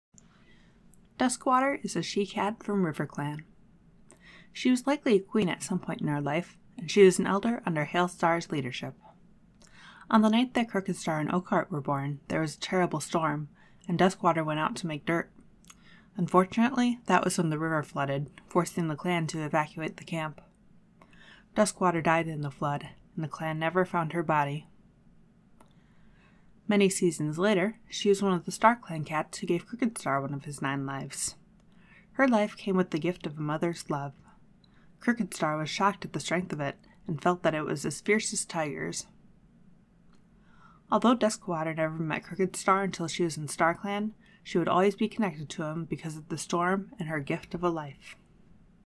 The Life of Duskwater
This week's warrior of the week is Duskwater, a character from Crookedstar's Promise who died in the prologue and appeared later in StarClan. Her physical appearance isn't described in the book, only through Vicky's Facebook page, but I picture her as dusky brown due to her name, kind of like Mousefur.

May contain minor spoilers for Crookedstar's Promise.